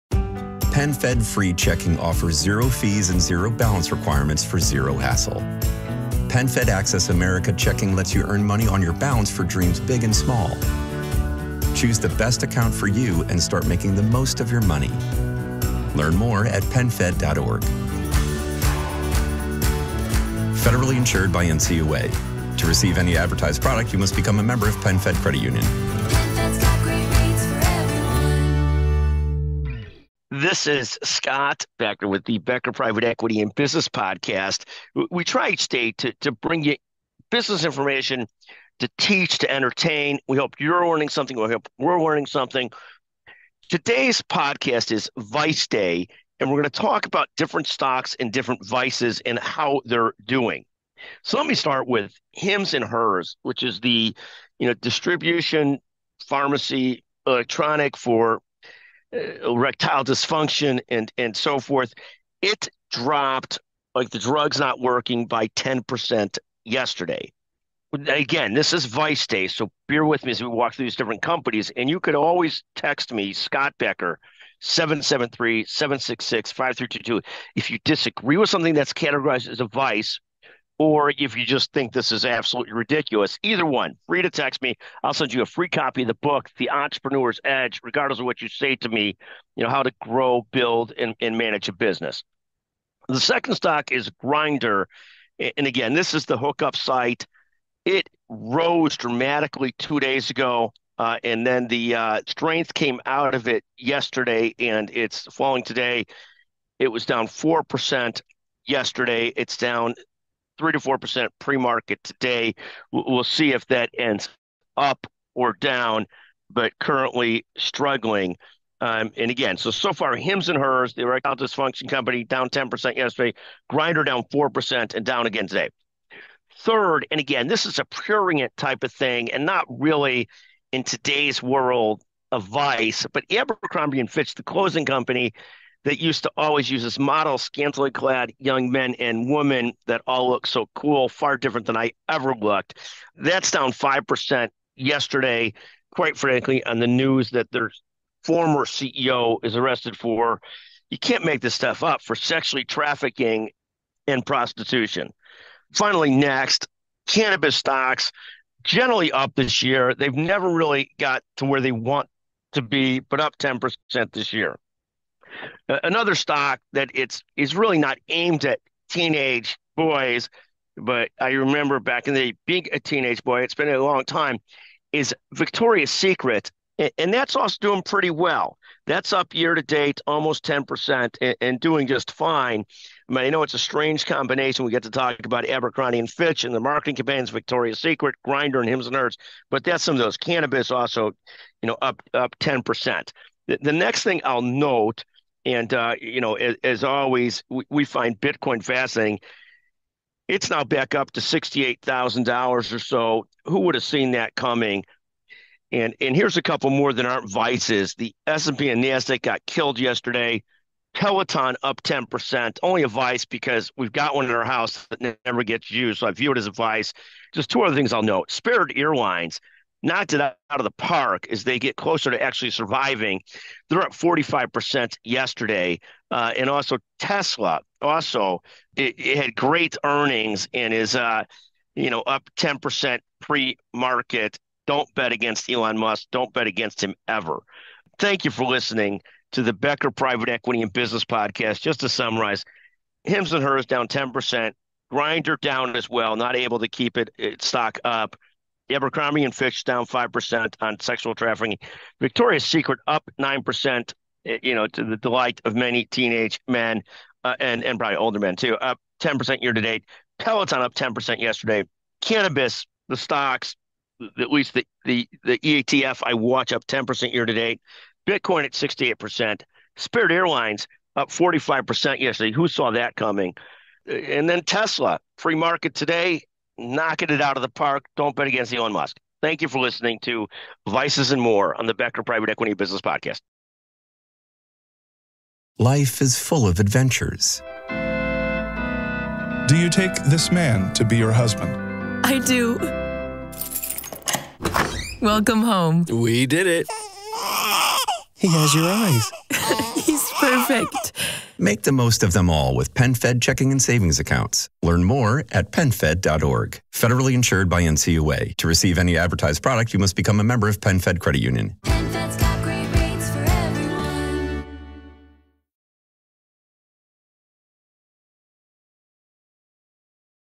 Insights on “Vice” Stocks and Surprising Market Shifts 10-24-24
In this episode, Scott Becker delves into the performance of various “vice” stocks, including Hims & Hers, Grindr, Abercrombie & Fitch, and cannabis. He also touches on the surprising rises of Spirit Airlines, Peloton, and Bitcoin, along with Tesla’s continued market dominance.